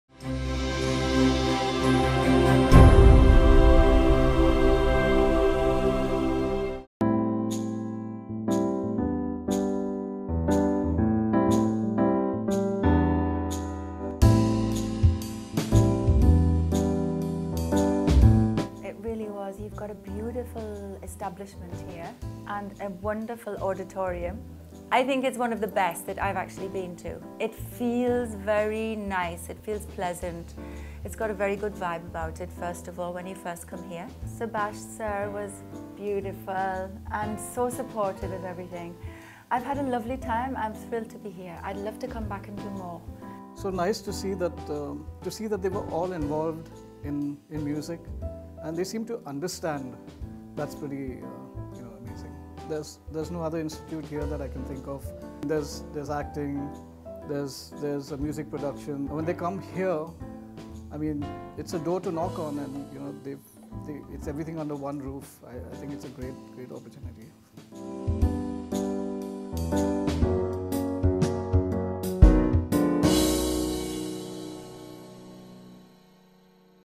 5th Veda Session with Dee Wood, Karim Ellaboudi, Jan Graveson & Adrian D'souza | WWI
Veteran jazz musician and faculty, Dee Wood along with his band members - Karim Ellaboudi, Adrian D’souza and Jan Graveson led an informative & musical session during the 5th Veda at Whistling Woods International, highlighting the intricacies of the art.